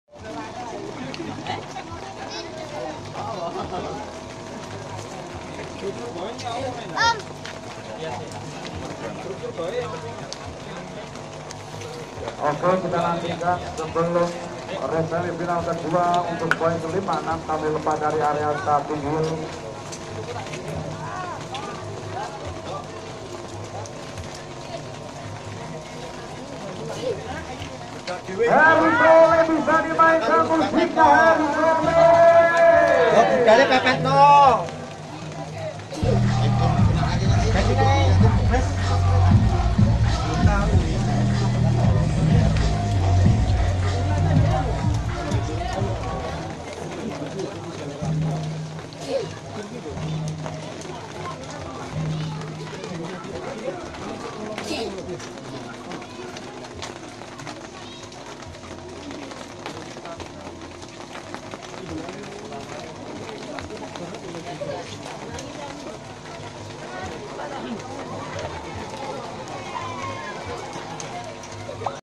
Balap Sepeda Jogja BMX Racing Supercross at Youth Center Circuit 2016
BMX Racing Supercross at Youth Center Circuit
Jogjakarta Bike Racing BMX Supercross Series
13 maret 2016 di Youth Center Circuit ,Cebongan Sleman Yogyakarta Indonesia.

BMX Racing
Youth Center Circuit